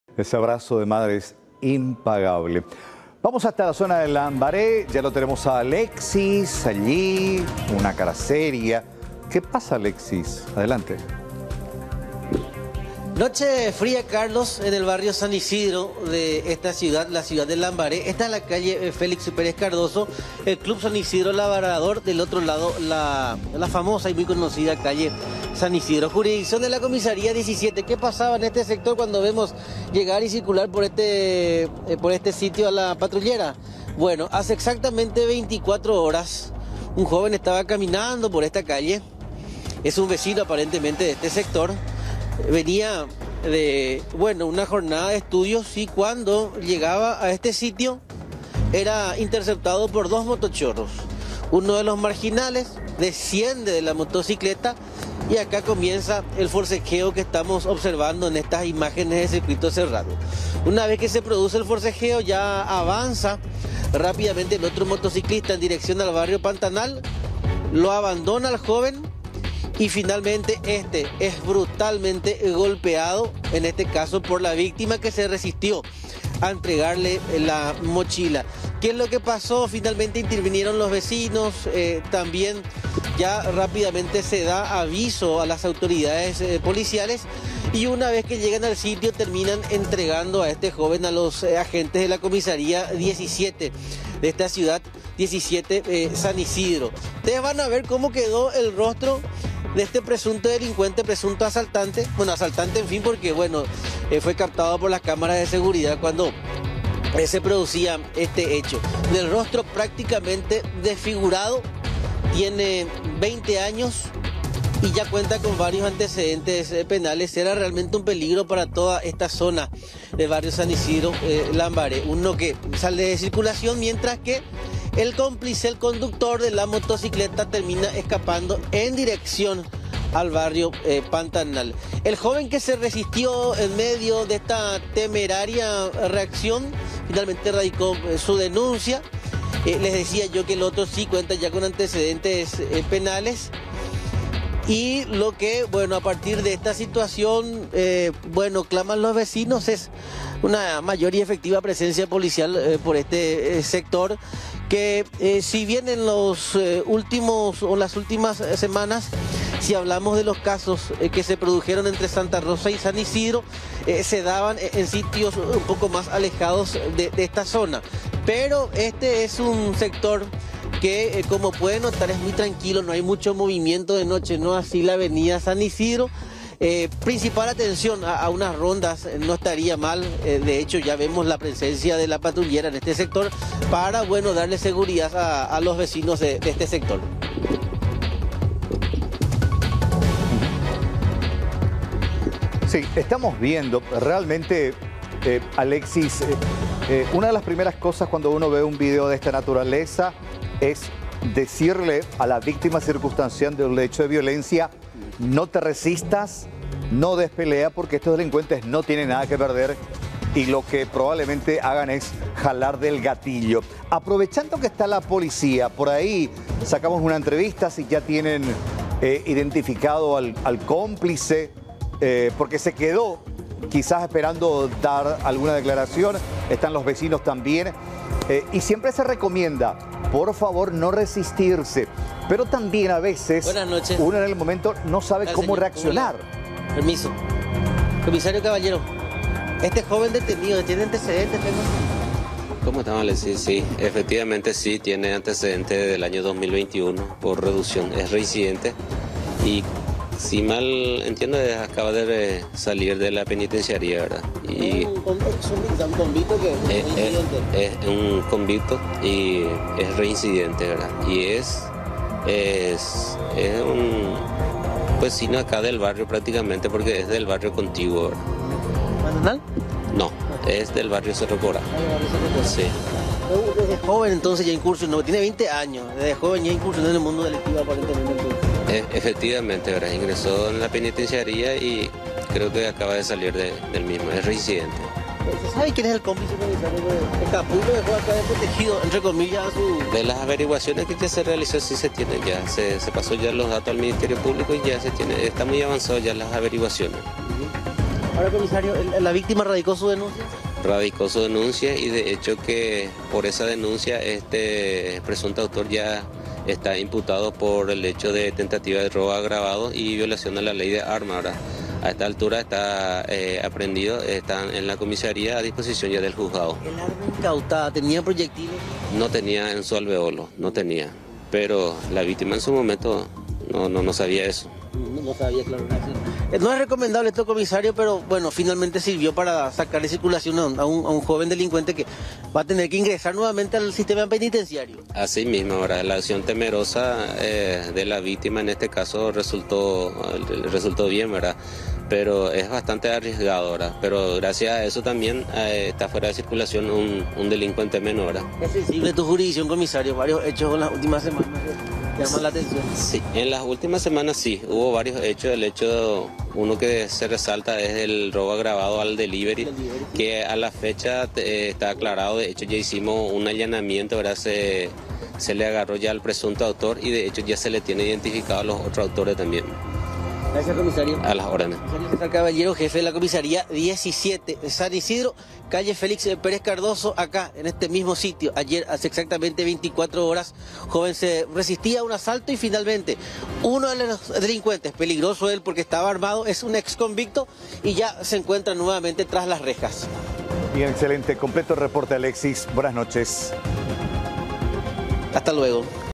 Peleó cuerpo a cuerpo con un asaltante
Tras una dura lucha, un joven logró reducir a su asaltante, en la zona del barrio San Isidro de Lambaré.

El detenido ya estuvo en la cárcel.